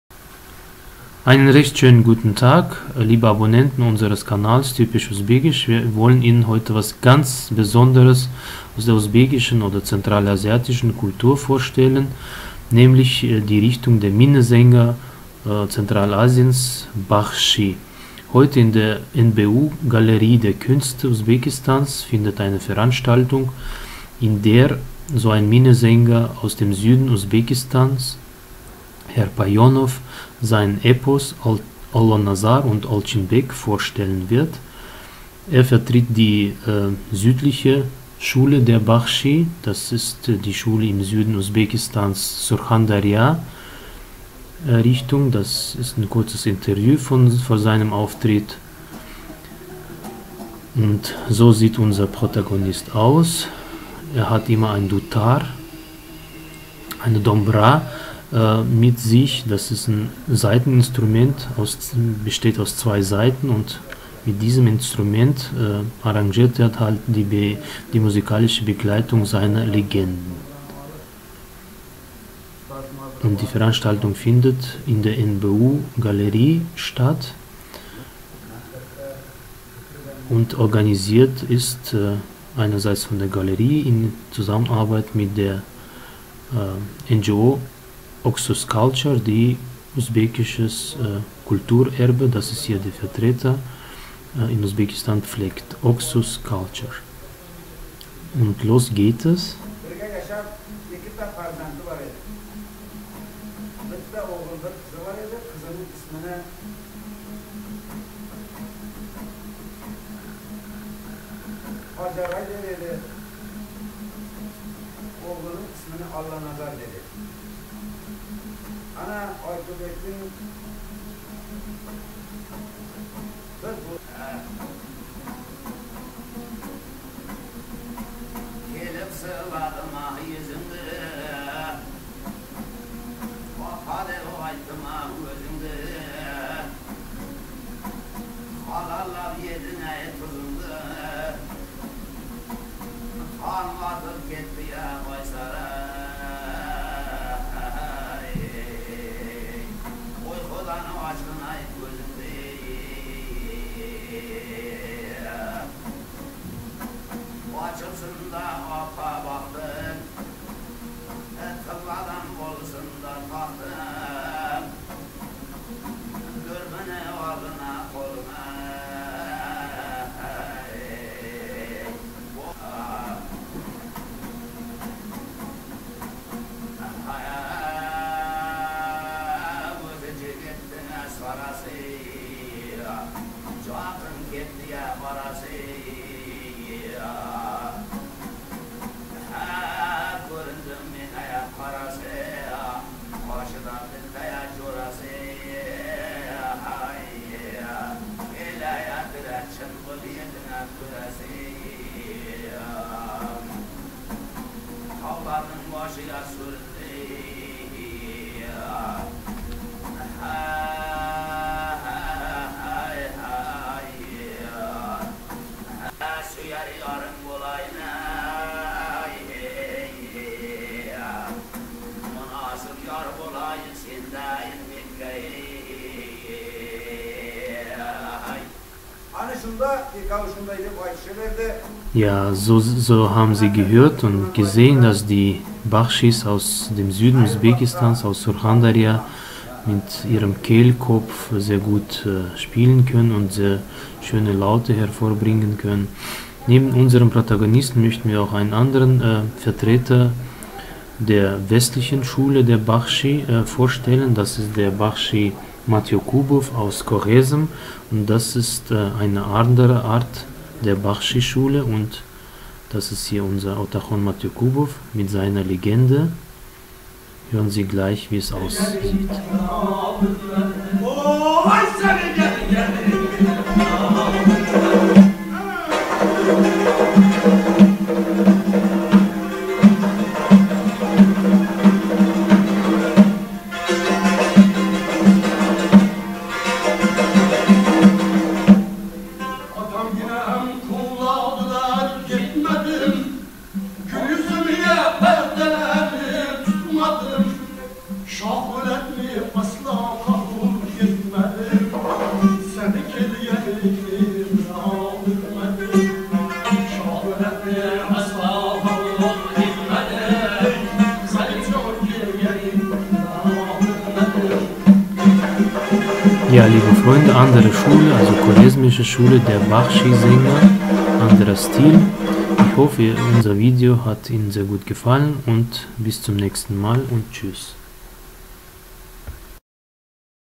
Bachschi Minnesänger Usbekistans
"typisch usbekisch" stellt Ihnen unikales Videos vor. Es geht um Bachschi-Minnesänger Zentralasiens. Das sind Sänger und Legendenerzähler. Sie sind willkommene Gäste während Volksfesten und Großfamilienfesten. Ihre Sagendichtung begleiten sie mit Musik, von dem Saiteninstrument "Dutar" (hat zwei Saiten). Die bekanntesten Epos Zentralasiens sind Manas, Alpomisch, Gerogli.